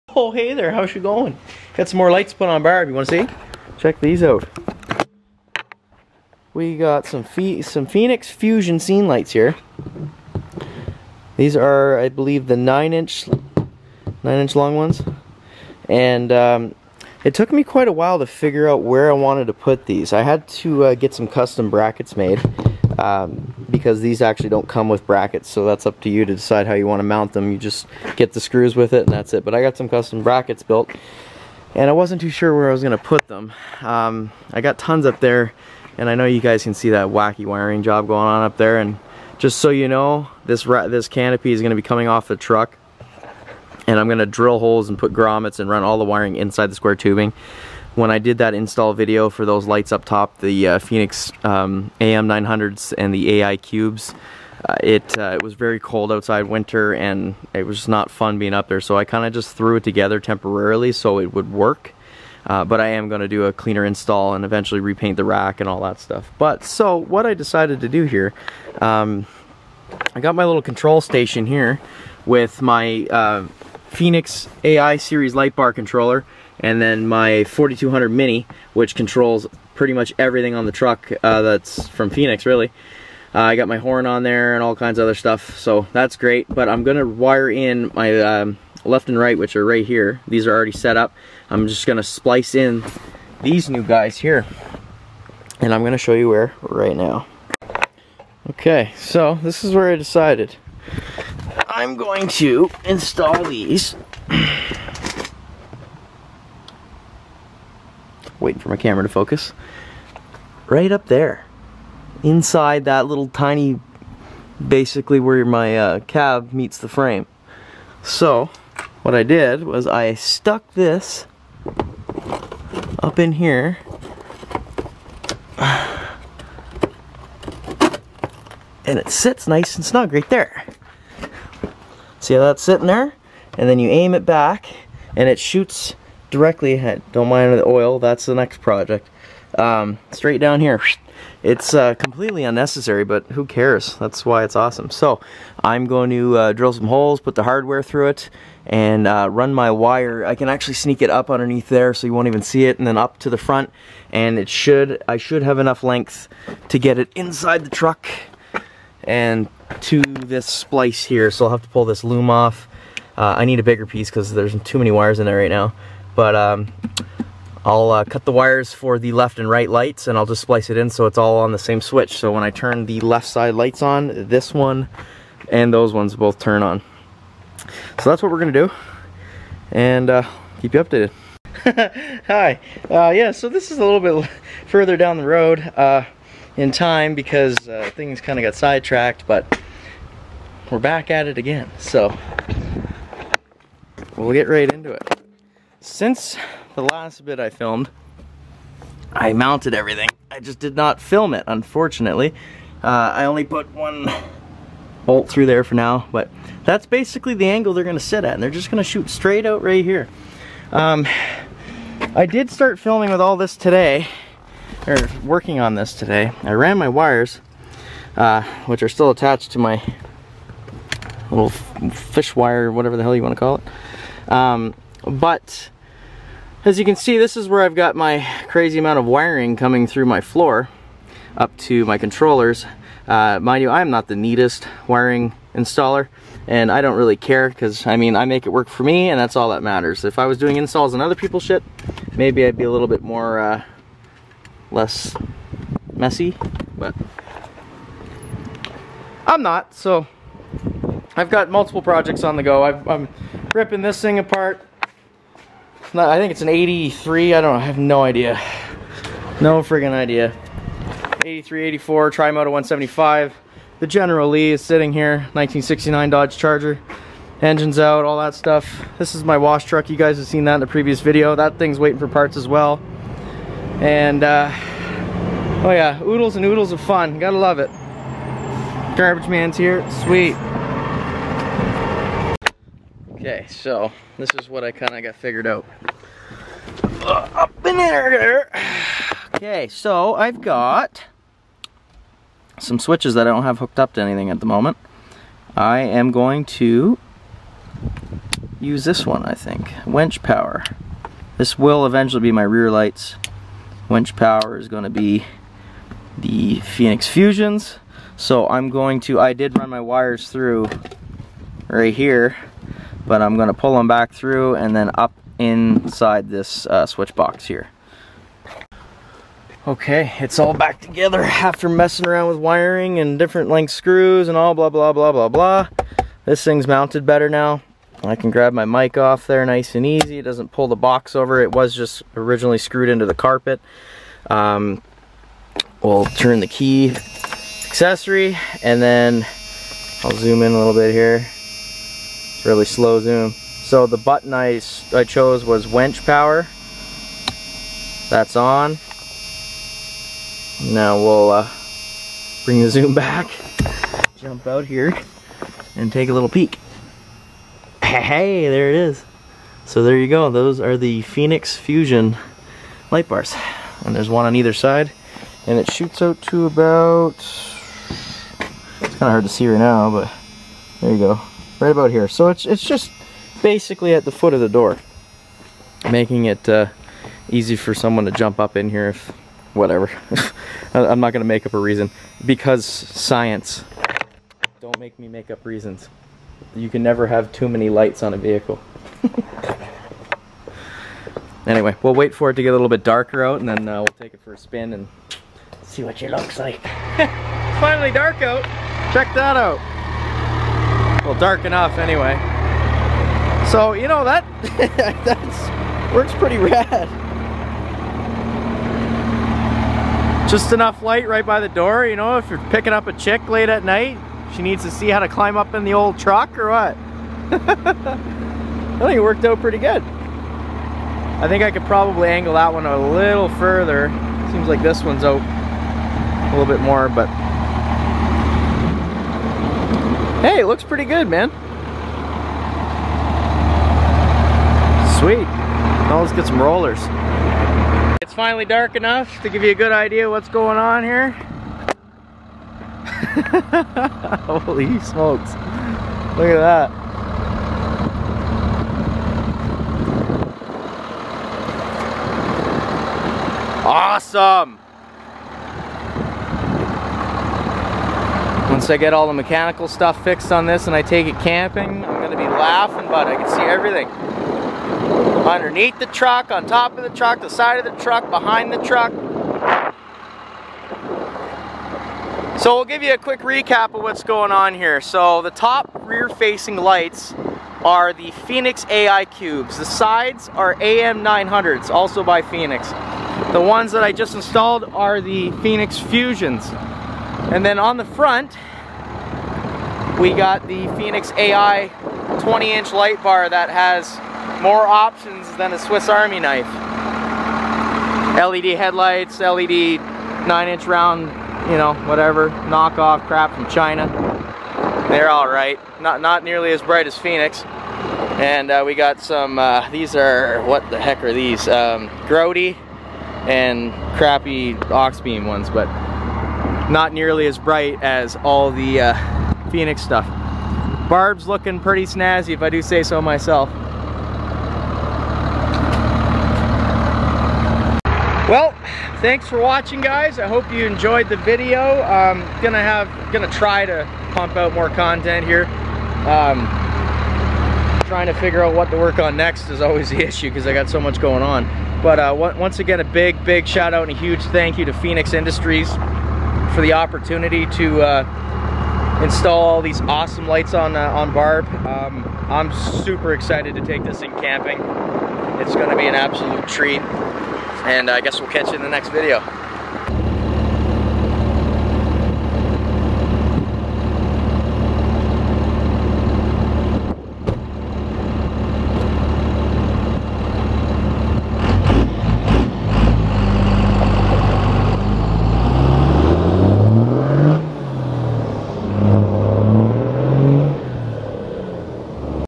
Last Light install on Barb
in this video i install the last batch of lights from Feniex. The Feniex Fusion 9.5in light bars